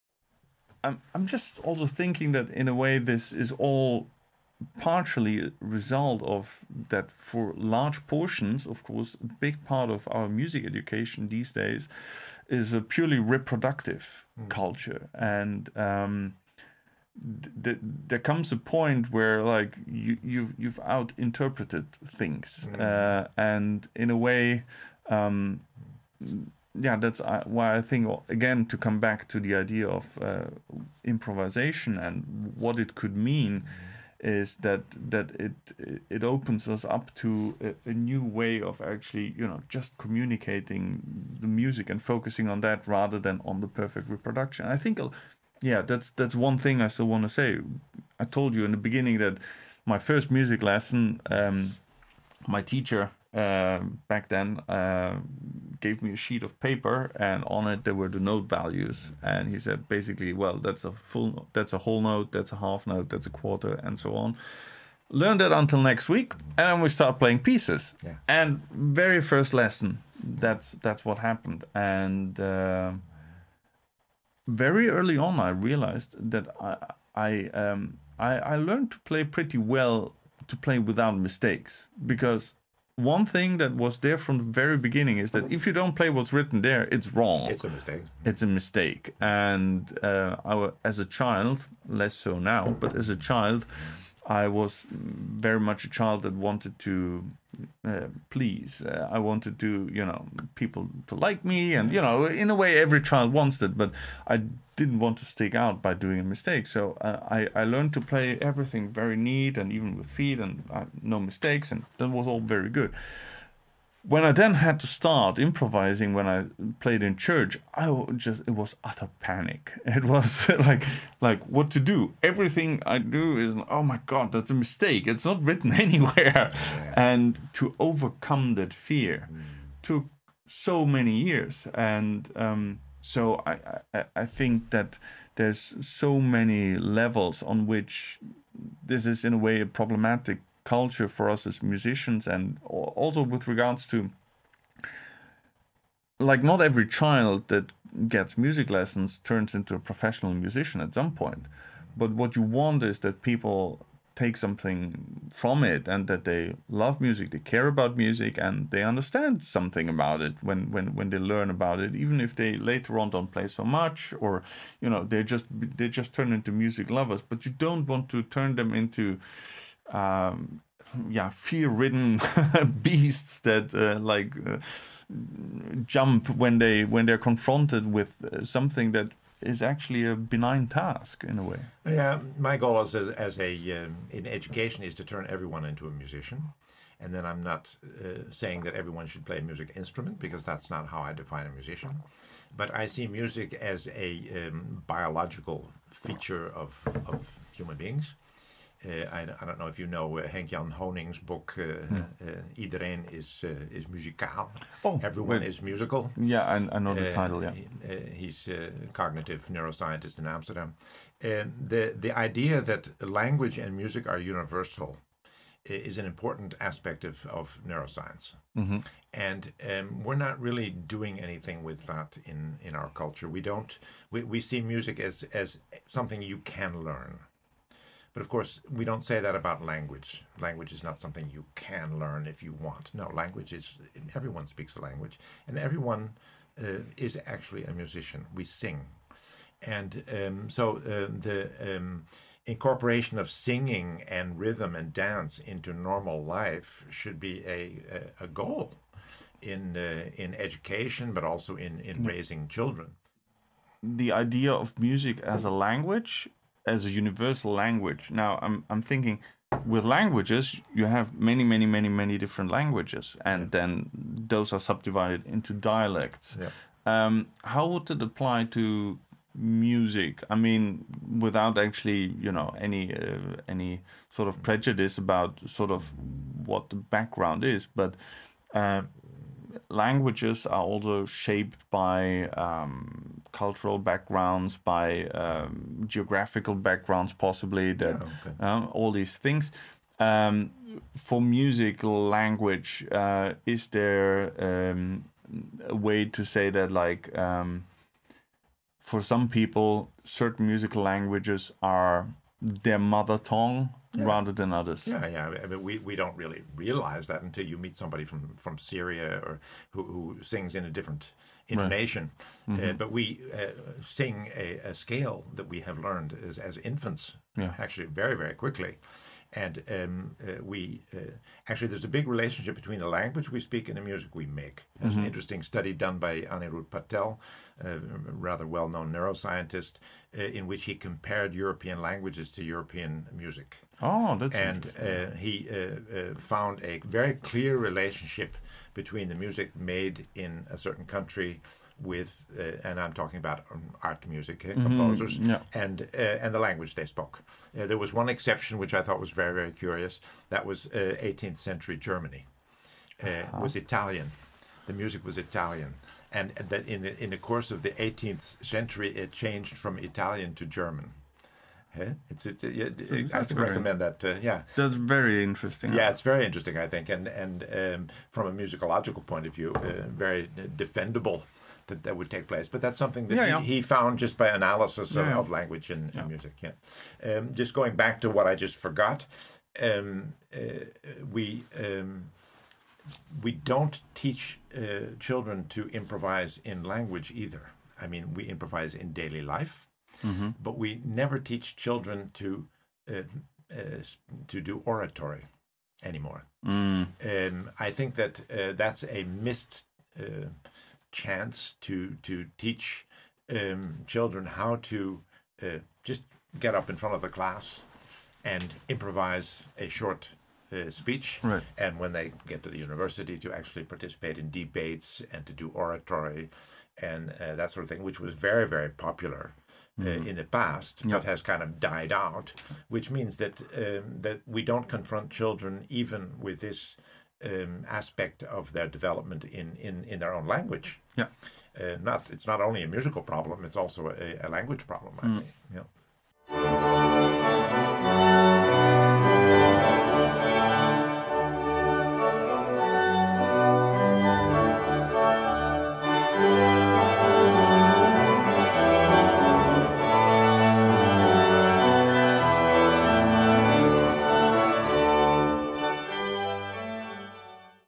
David Boos & Robert Harris on reproductive culture & musical mother tongues – DoD#1
Henkjan Honing: Iedereen is muzikaal

Aniruddh Patel: Music, Language, and the Brain